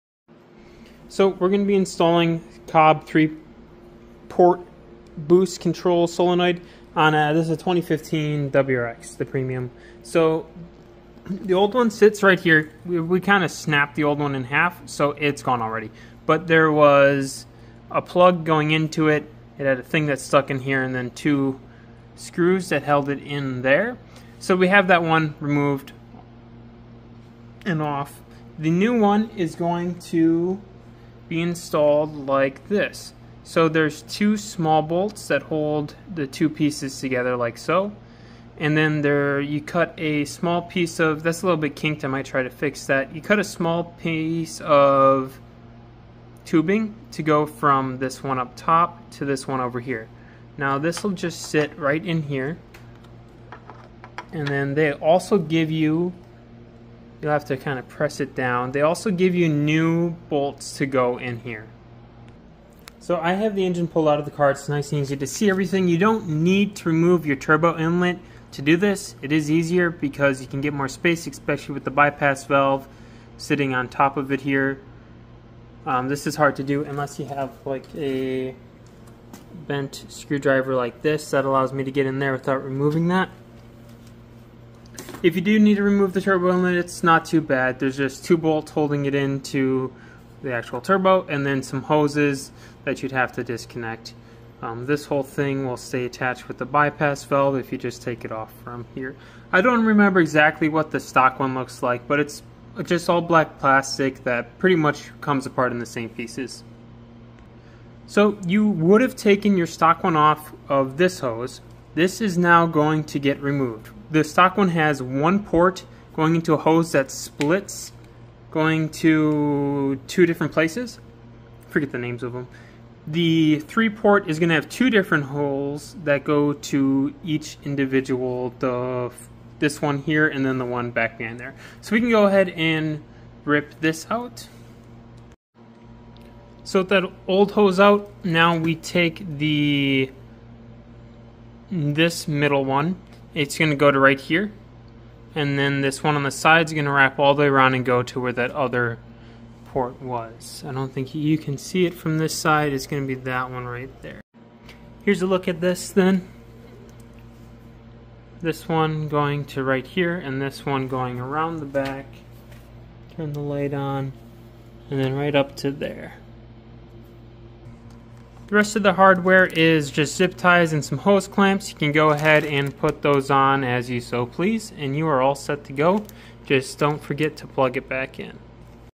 Turbocharger Upgrade - Cobb 3-Port BCS Install - 2015-2021 WRX
This video will show you how to install a Cobb 3 Port Boost Control Solenoid on a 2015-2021 Subaru WRX.